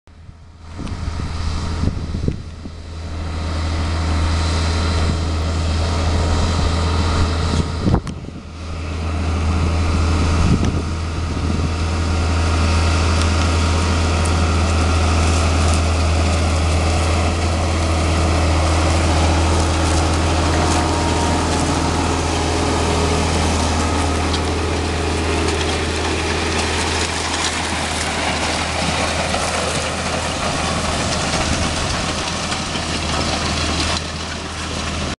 Lackham EDFM 2011-2013 24 Hour Ploughing Marathon 2
A Fendt 936 with an 8 Furrow Wagon Plough with a Packer Mat